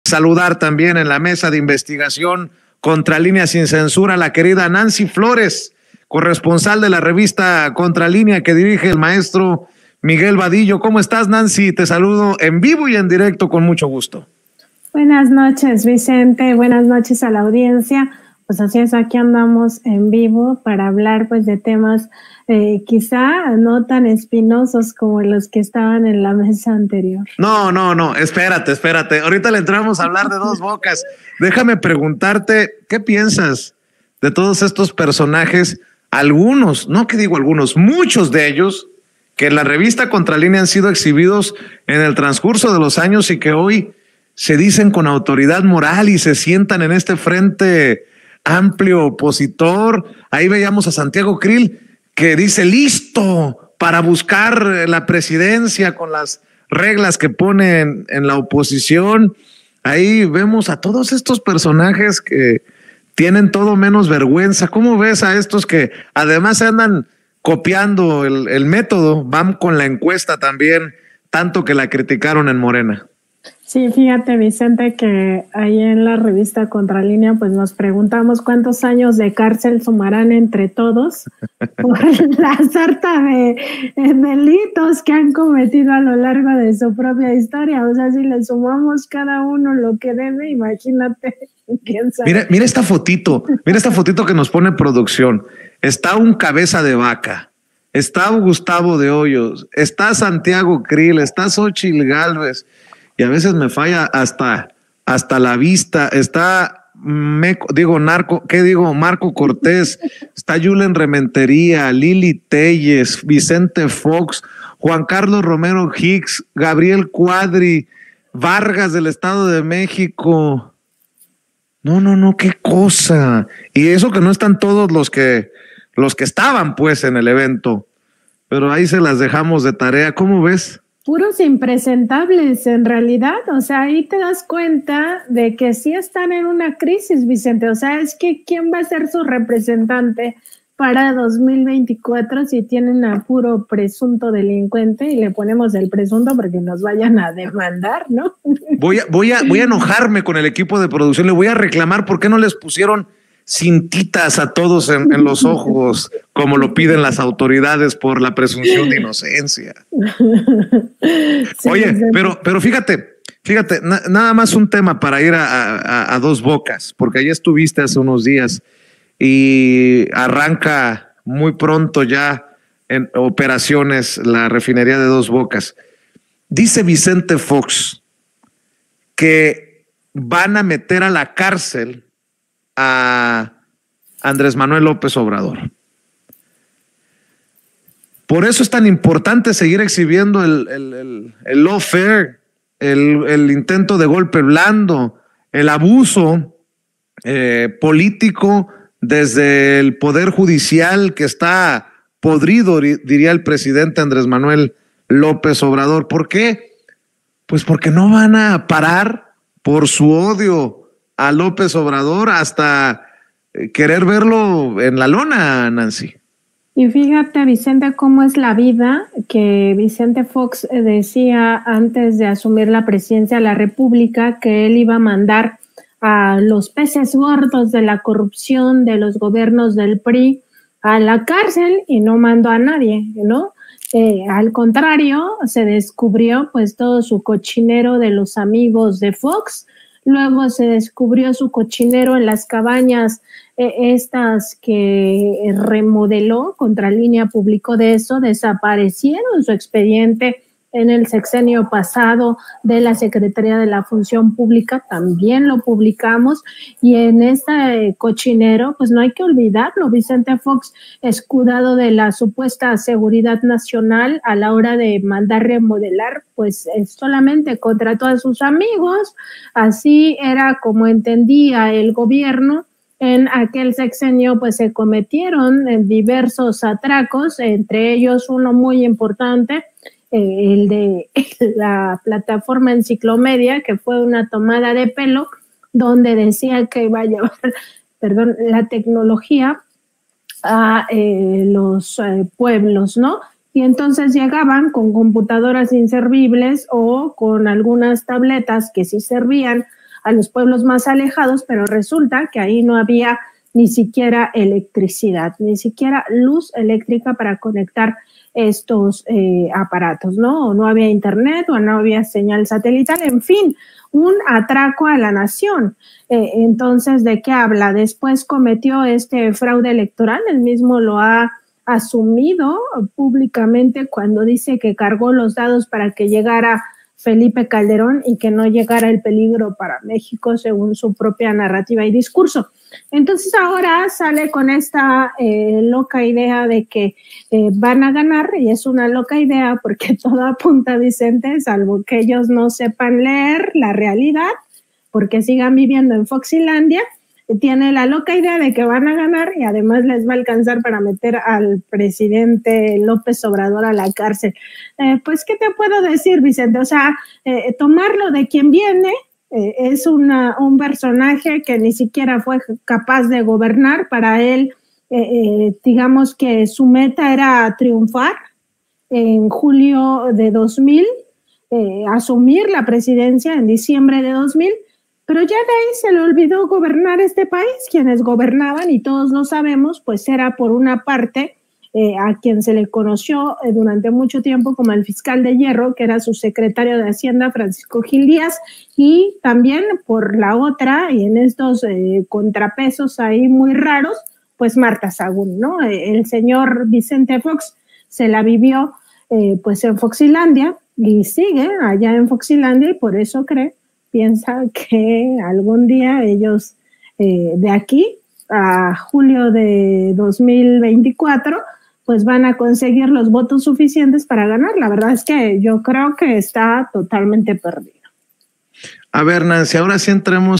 ANTES DE AMENAZAR A AMLO ¿CUÁNTOS AÑOS DEBEN CRIMINALES POLÍTICOS DE DERECHA?: NANCY FLORES
La periodista de Contralínea Nancy Flores aseguró que los políticos de la derecha deberían sumarles los delitos y ver cuántos años de cárcel deben antes de amenazar a López Obrador